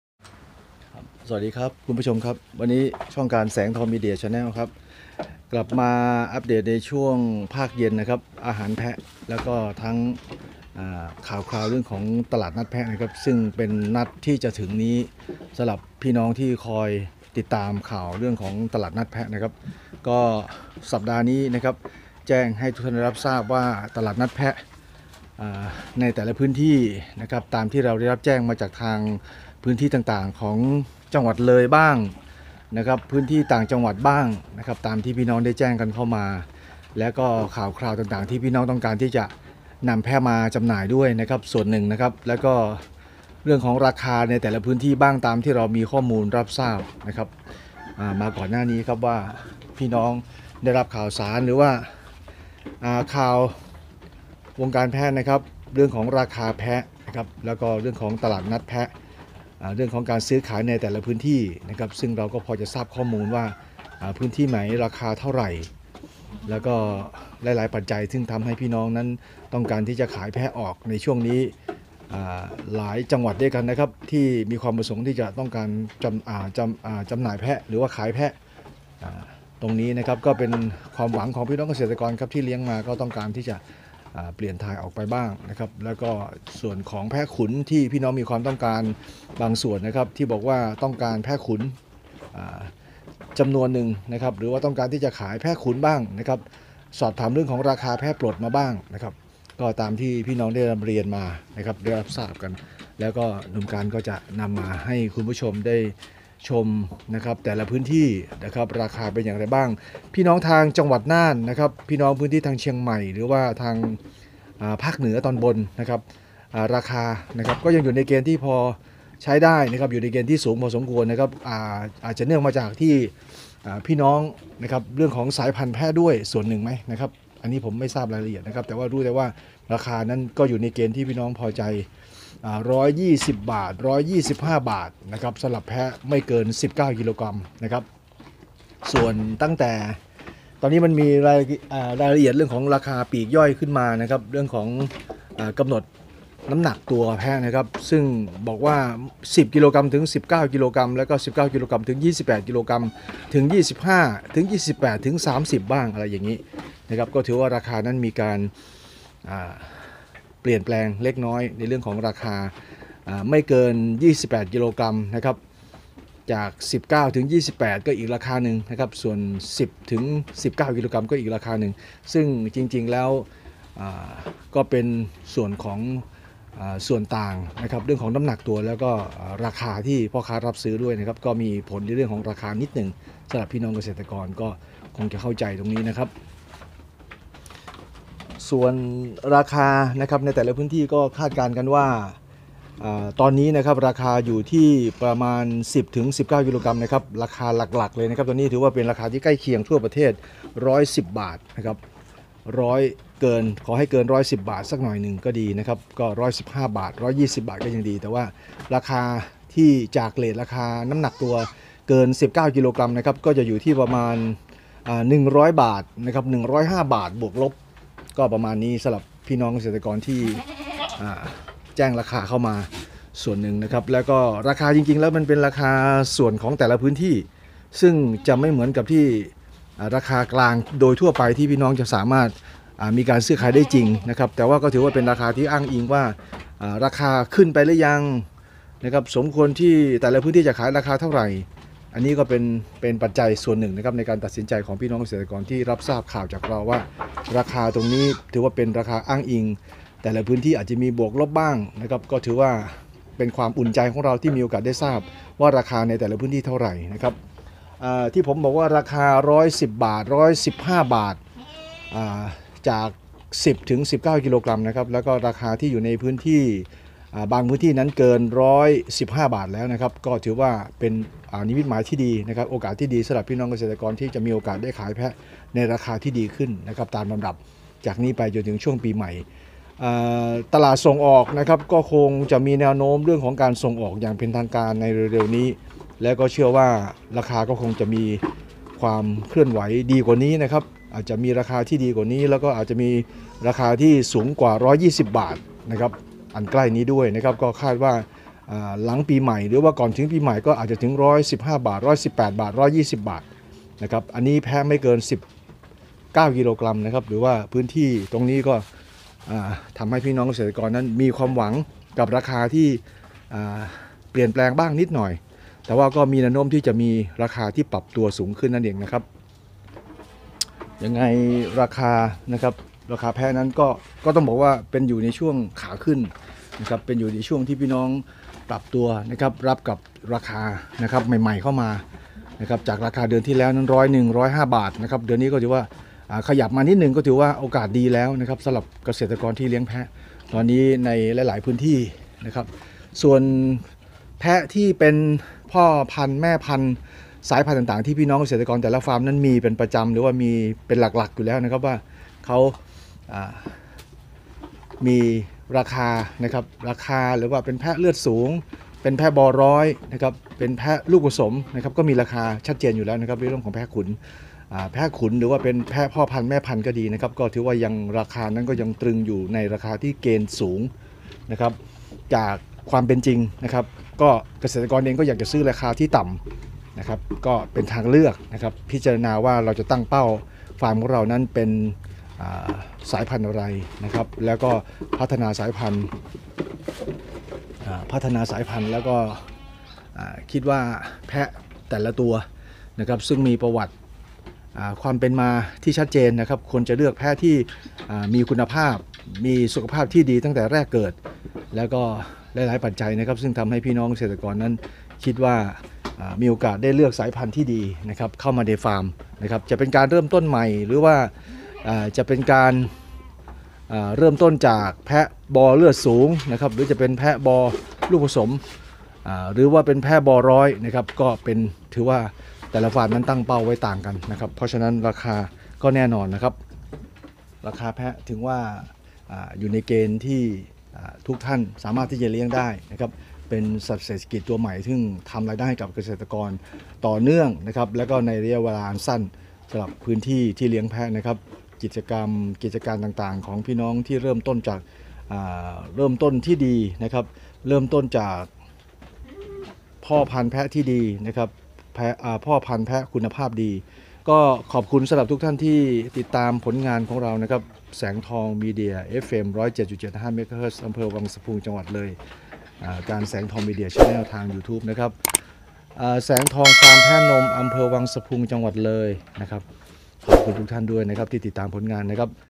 ตลาดแพะมาแรงราคาปรับขึ้นส่งท้ายปี! ราคาแพะเนื้อแพะขุนมีปรับราคาก่อนสิ้นปี.
ราคาแพะเนื้อแพะขุนปรับราคาแรงส่งท้ายปี รองรับตลาดแพะส่งออกต่างประเทศต้นปีหน้า
แรงต่อเนื่องราคาปรับขึ้นตามกลไกตลาดโลก